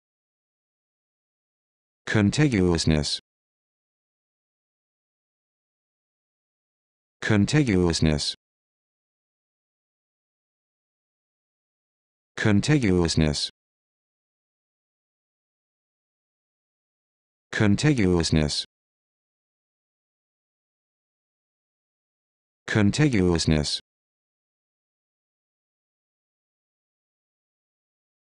How to Pronounce Contiguousness
This video shows you how to pronounce Contiguousness